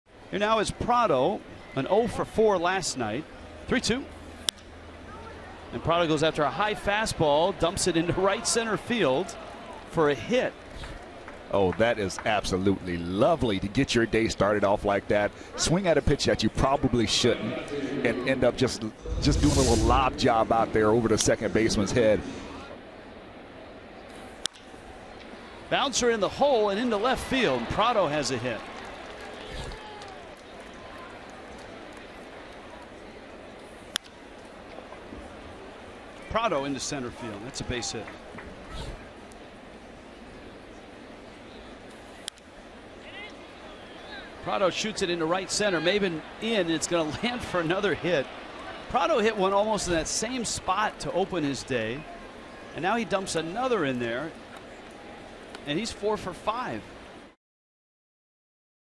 MIA@DET: Prado notches four hits against the Tigers
Martin Prado tallies four hits in five trips while also scoring one run vs. the Tigers